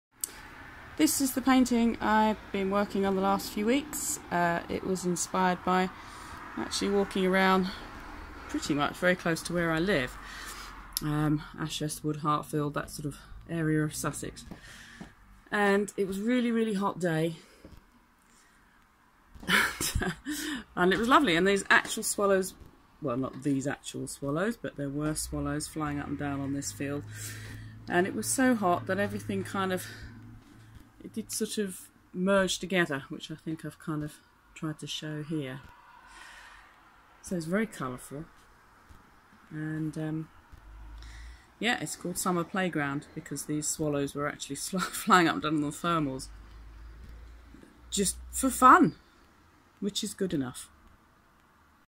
Video about the painting Summer Playground by Gill Bustamante
Summer Playground is a large semi abstract oil on canvas of a summer meadow with wildflowers and swallows. I painted it after a walk through fields and farmland between Hartfield and Ashurstwood in Sussex on a very hot July day. There was wheat and barely and corn growing in the fields and at the edges there were a myriad of wildflowers growing. Insects were buzzing and the birds were singling and calling in the trees of the hedgerows and patches of woodland. One particular meadow had swallows flying up and down incredibly fast using the thermals which made them even faster than they usually are. They were not doing it for any obvious reason other than it was fun I think. I am happy at the way the colours on this painting merge and seem to melt together slightly as it is reminiscent of the heat of this day and has captured it well for me. I can hear the birds and the insects, smell the crops and the flowers and remember how happy it makes me feel. England in summer is fleeting and sunny days are a minority so it is good to catch them when they occur which I think I have done.